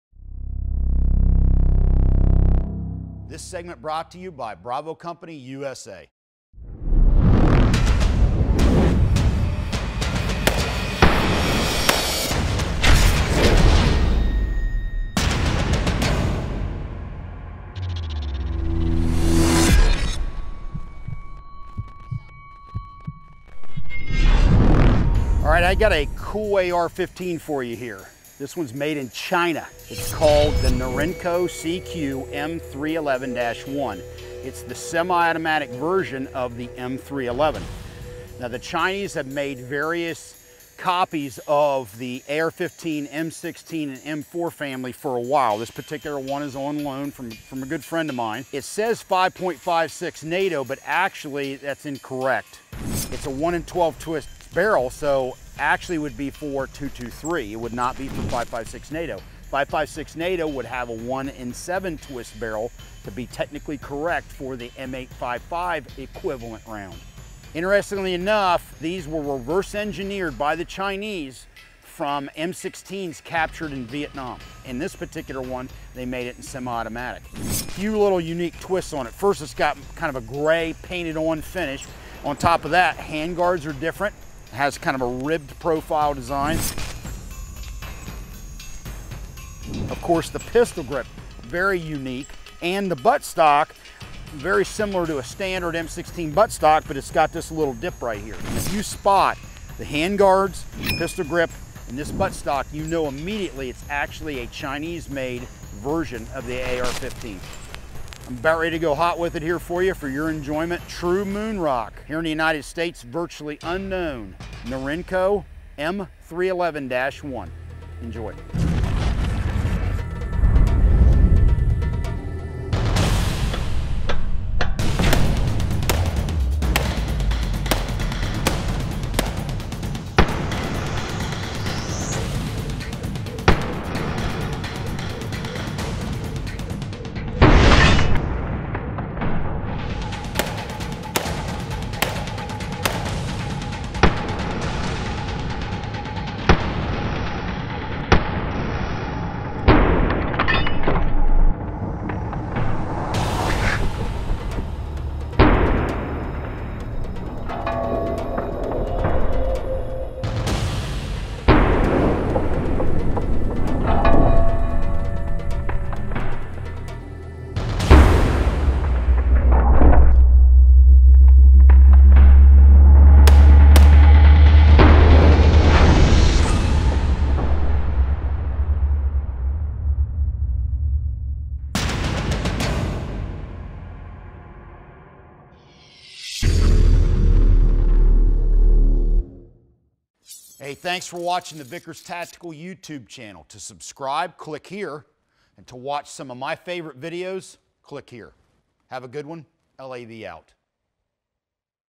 Chinese AR-15 - Norinco CQ M311-1
I got a unique chance to shoot a Chinese AR-15 variant the Norinco CQ M311-1. We were very lucky to get one for the channel, hope you enjoy the video.